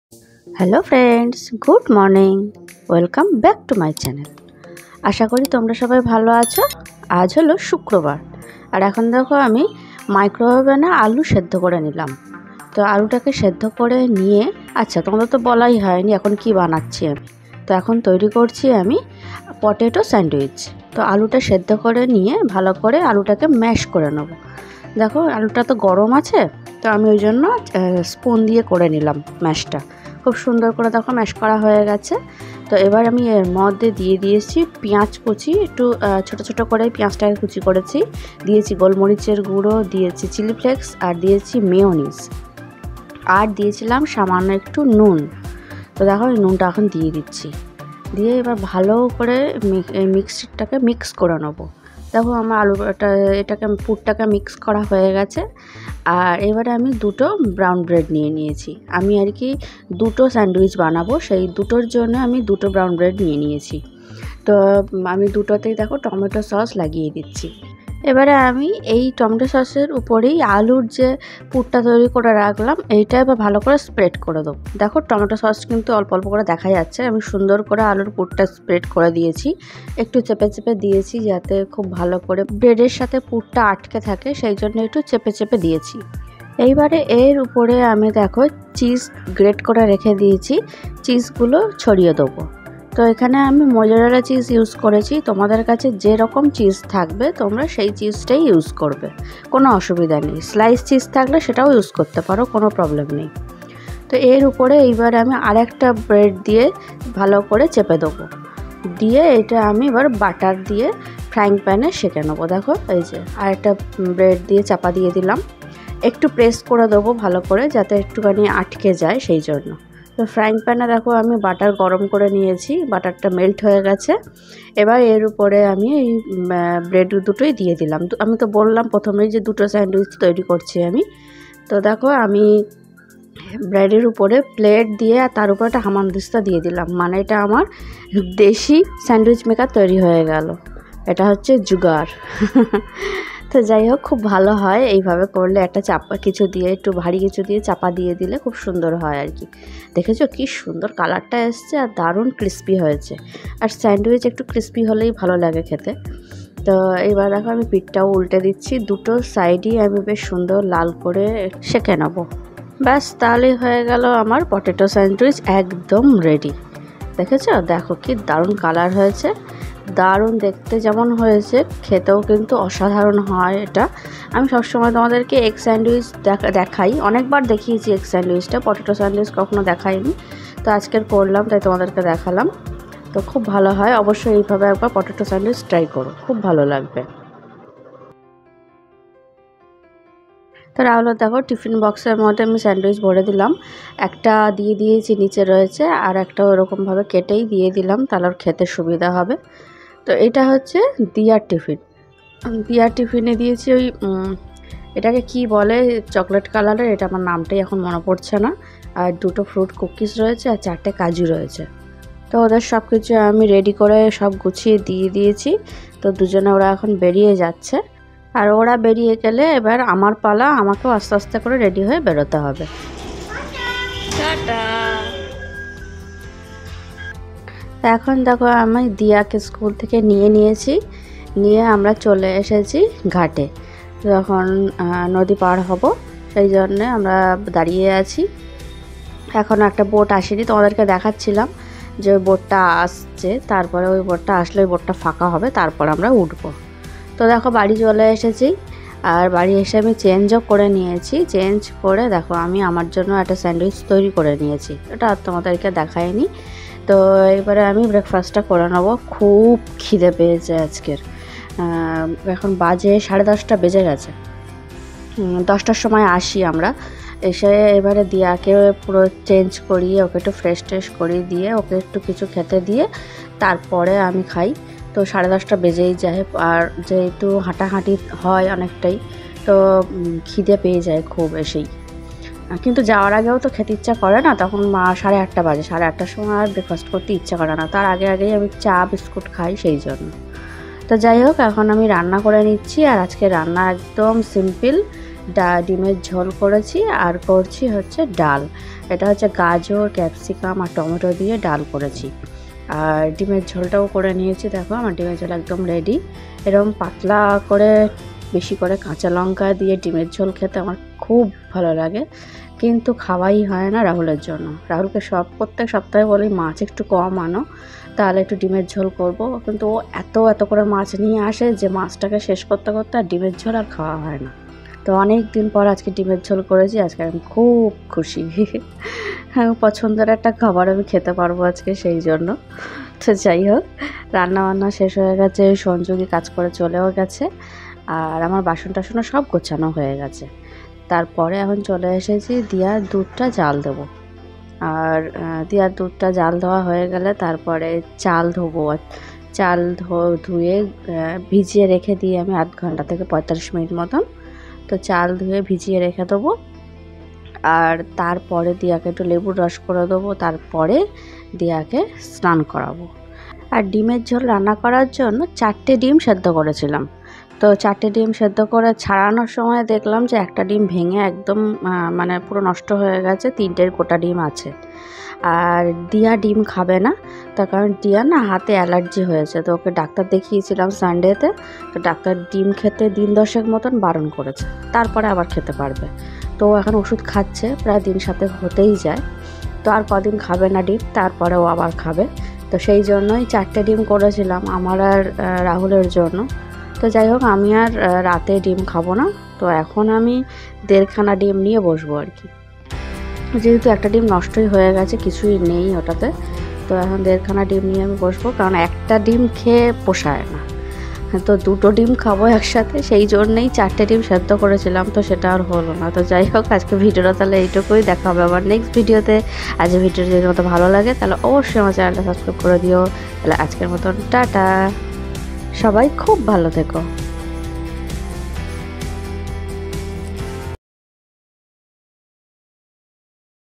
আজ একটা দারুন ব্রেকফাস্ট আলু স্যান্ডইচ করলাম।। potato sandwitch recipe
bengalivlog

How spend a day with my baby girl. My cooking.